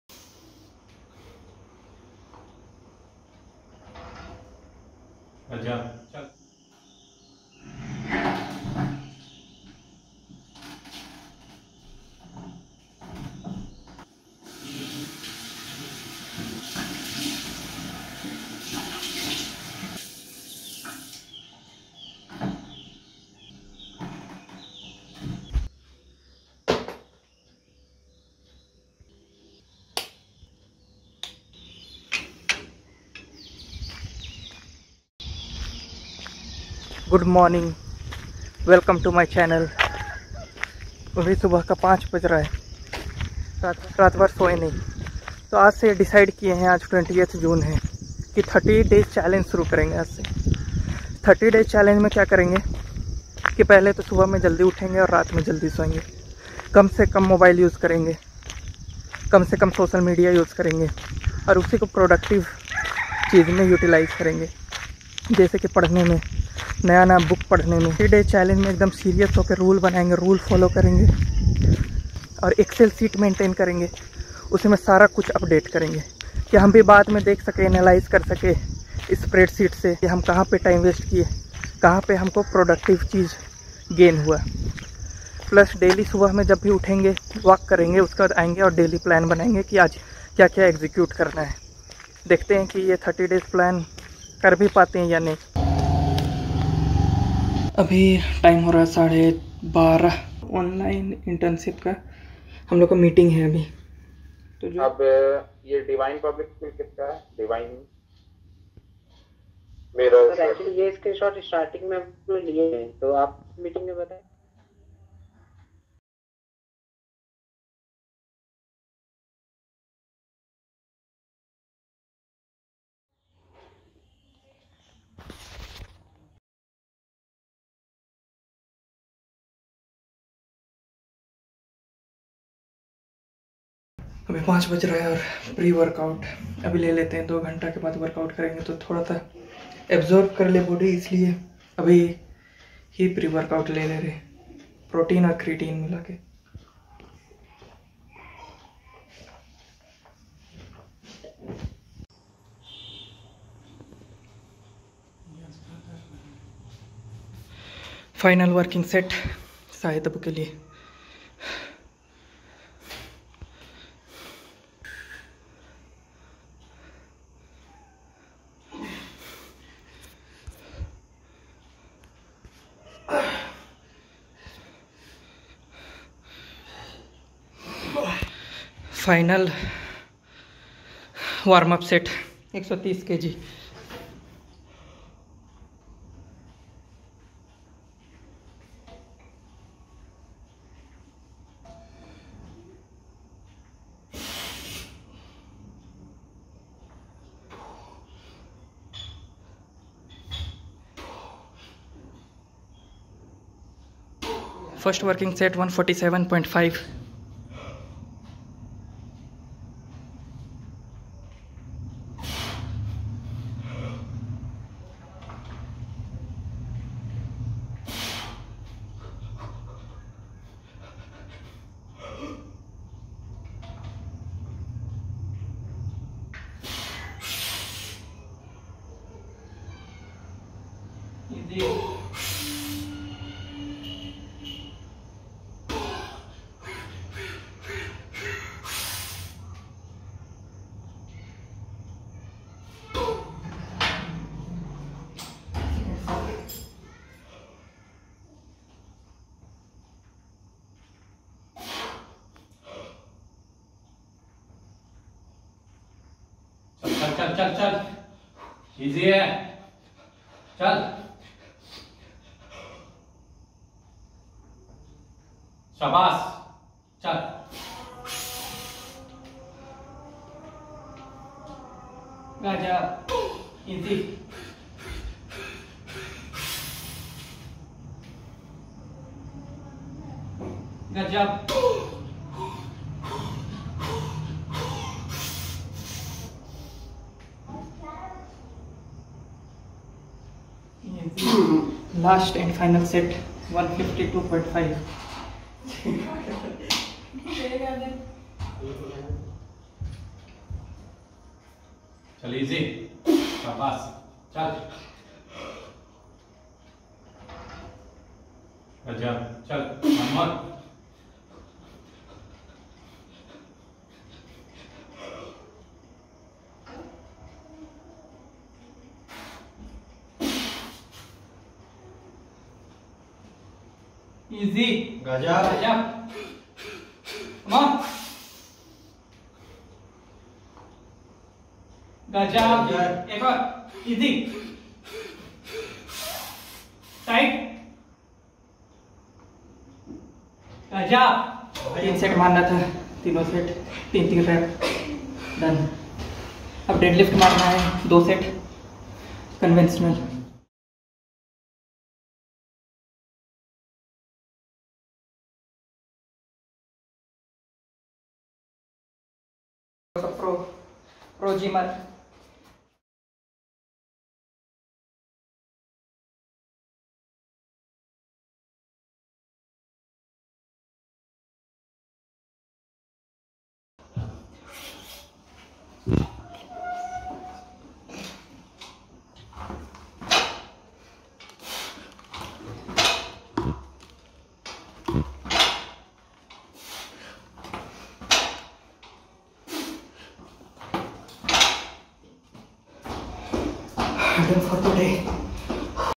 DAY 1 | 30 DAYS CHALLANGE | NO SOCIAL MEDIA | BHARAT BAND | SQUAT | GYM | TRADING | HALF TANK
Today was bharat band.
Day 1 of 30 days productivity challenge.
No social media.
Heavy squat session.
Read book disciplined trader.
half tank